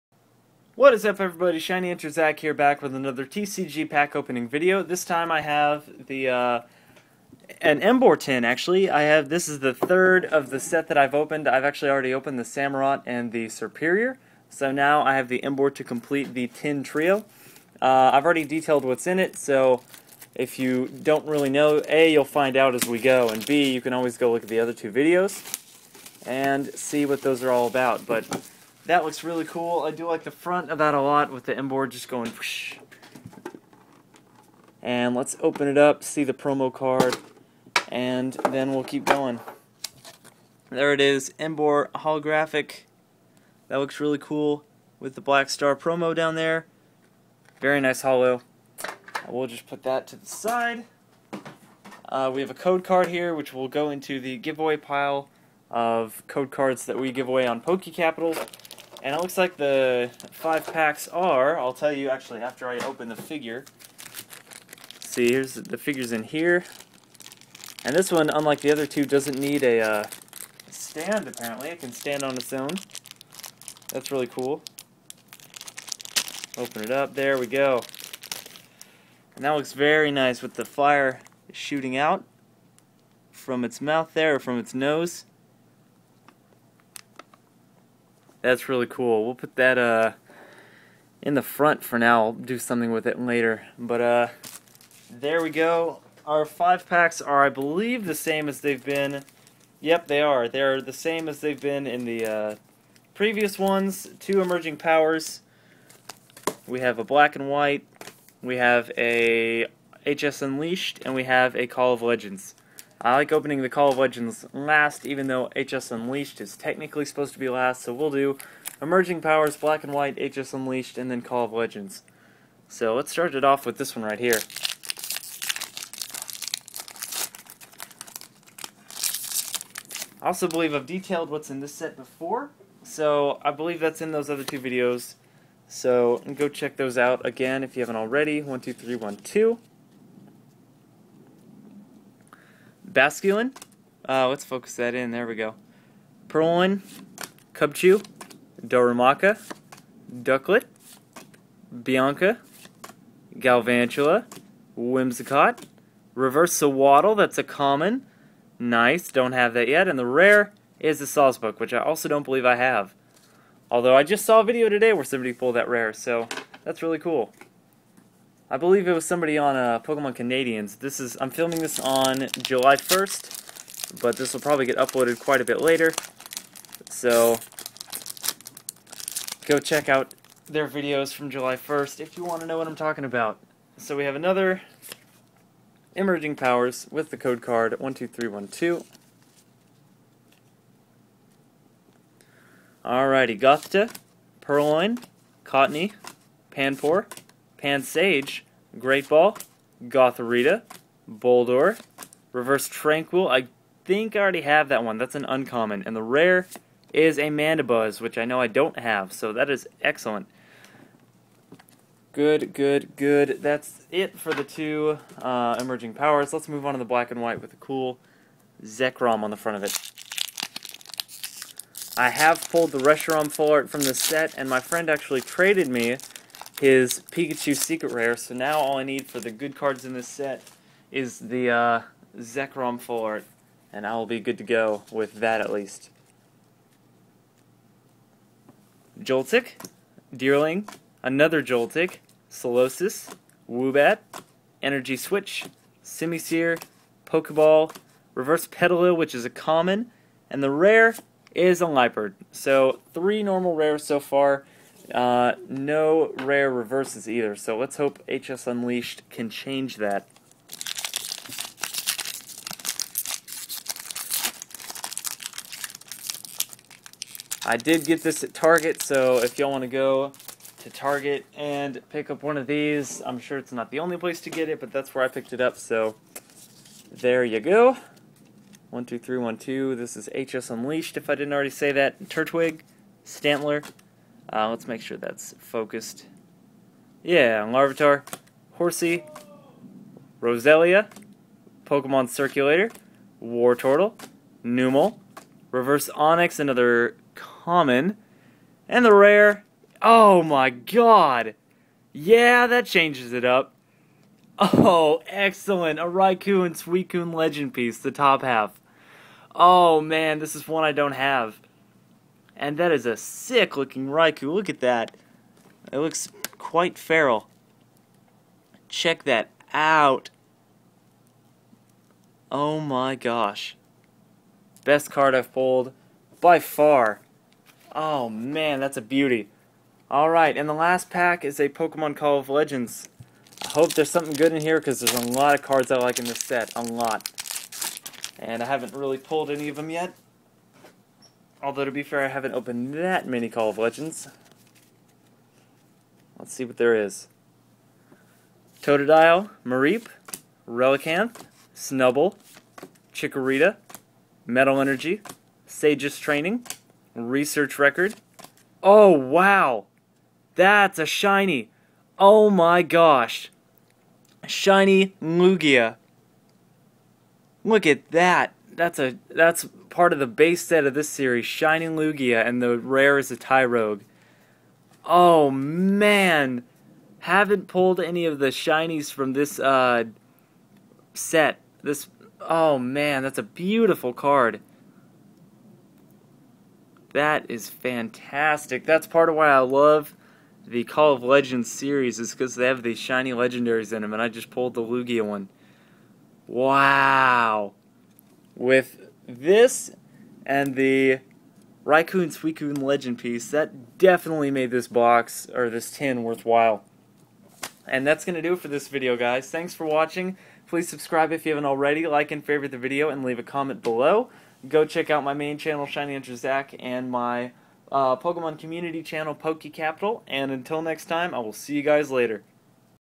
TWO GREAT PULLS! Opening an Emboar Tin!
Got a sweet reverse and an epic ultra rare.